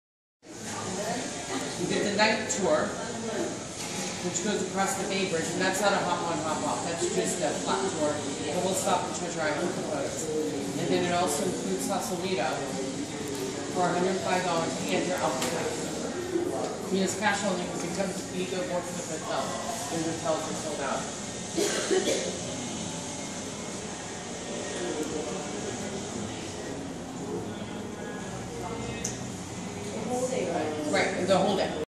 The Lobby of Pier 2620 Hotel Fisherman's Wharf San Francisco
A 4-minute walk from Fisherman’s Wharf, this upscale contemporary hotel is 2 blocks from the streetcar to the Embarcadero waterfront area. It's a 7-minute walk from the stores and eateries in Ghirardelli Square and 4.4 miles from the Golden Gate Bridge.